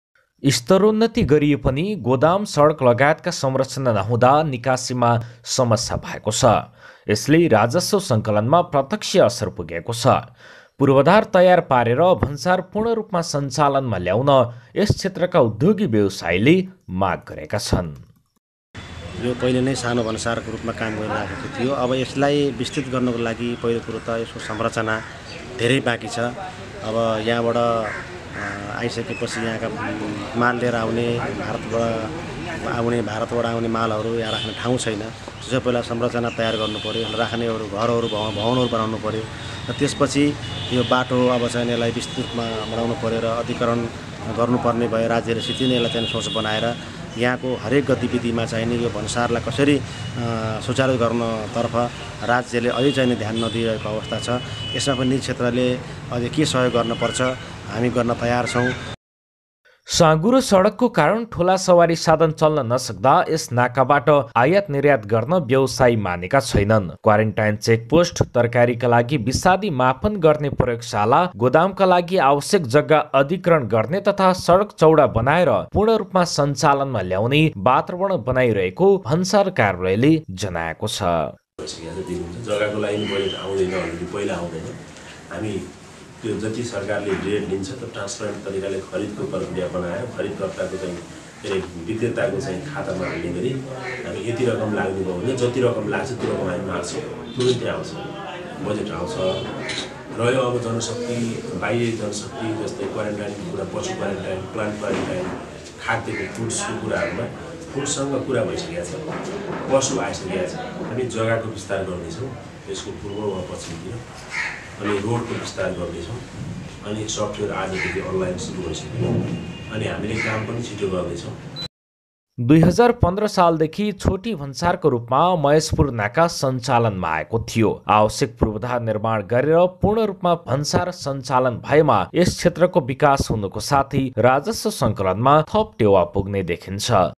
नवलपरासीको महेसपुर_भन्सार_कार्यालय
नवलपरासीको पाल्हीनन्दन गाउपालिका वडा १ का महेसपुर भन्सार कार्यालय
Twitter ©Take a look at महेशपुर न्युज नवलपरासी Maheshpur News NawalParasi (@MaheshpurNews)